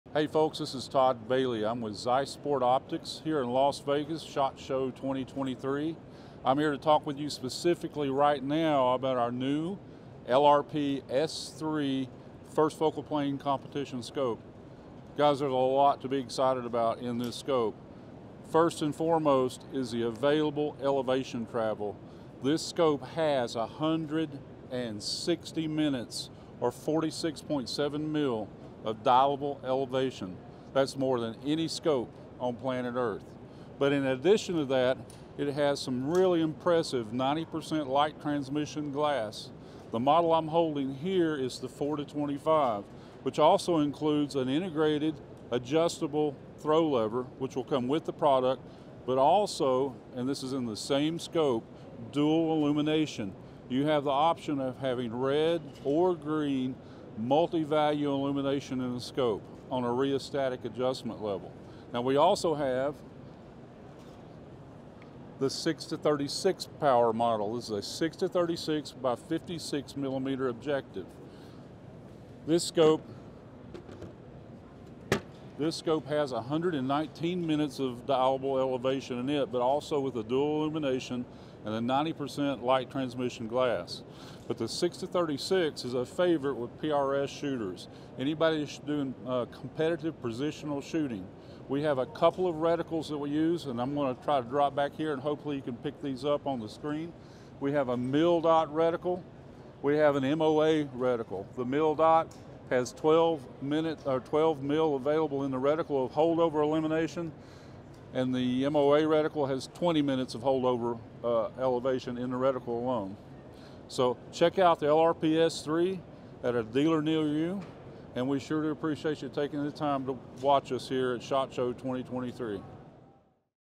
Quick look at the NEW Zeiss LRP S3!
We stopped in at the Zeiss booth at Shot Show to check out their new LRP S3 rifle scopes.